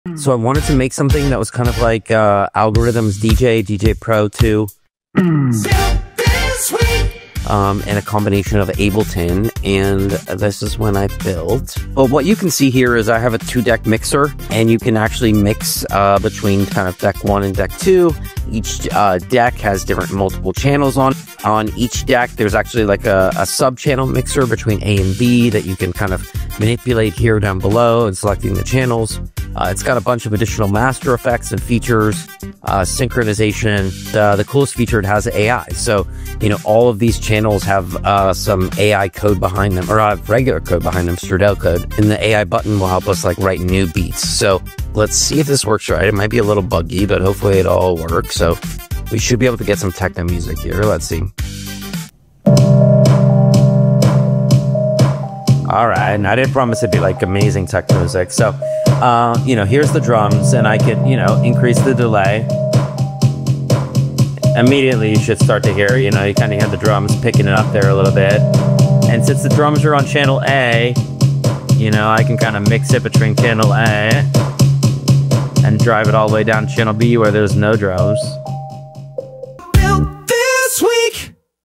AI DJ: Coding Beats Live with Strudel 🎶🤖 | Built This Week Podcast
What happens when you mix JavaScript, AI, and DJing? 🎧
Jordan Metzner from Ryz Labs built a live-coding DJ tool with Strudel, letting AI remix tracks on the fly.

This clip is from Episode 11 of Built This Week where we dive into:

🎶 AI + Music – coding beats with Strudel + AI

📊 Claude dashboards – CFO-ready insights from raw data

💻 OpenAI’s $1.1B StatSig acquisition and the coding wars